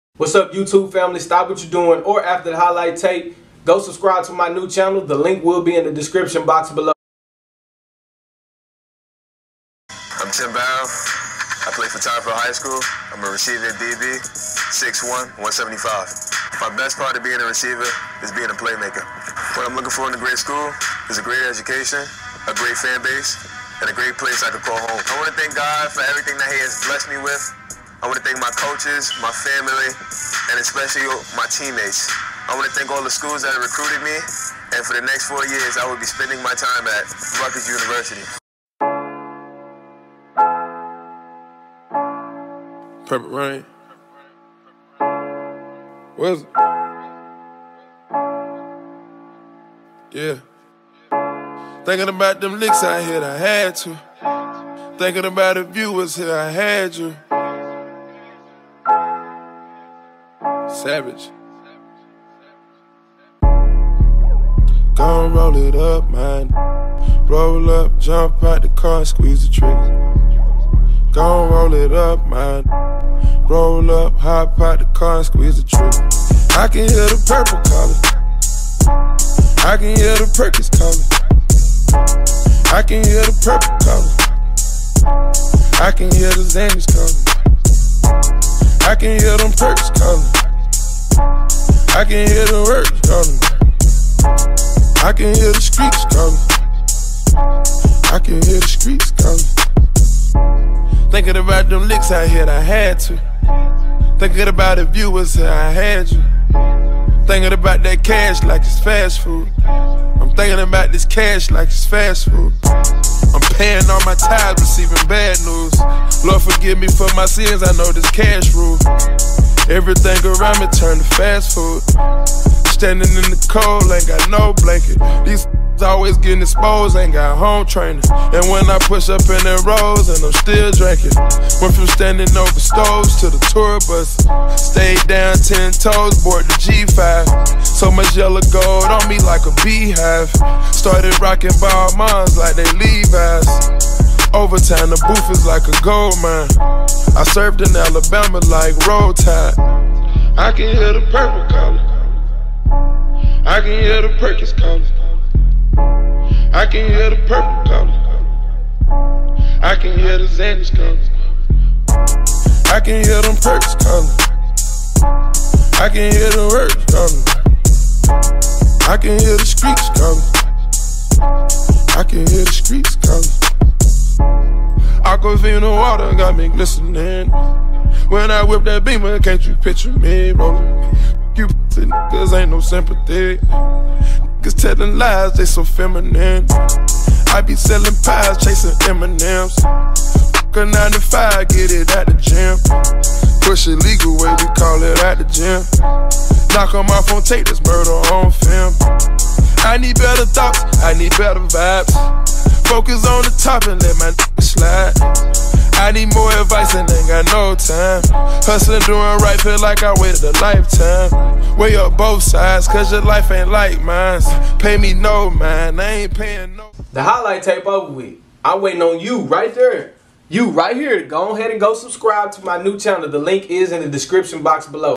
TIM BARROW || RUTGERS COMMIT
Back in February, Rutgers and head football coach Chris Ash offered Tottenville (NY) athlete Tim Barrow his first scholarship. Since then, he has added offers from Buffalo, Boston College, Monmouth and UMass and has seen his recruitment pick up.
However, Barrow, who is being recruited by Rutgers as an athlete, formally committed to Rutgers on Thursday after naming the Scarlet Knights his leader a few months ago.
The talented athlete caught up with 247Sports following his commitment."I choose Rutgers because it is close to me and I love the atmosphere there," Barrow told Rutgers247. "I love the coaching staff and I love what Coach Ash is doing to the program and how he is bringing the Ohio State culture to Rutgers."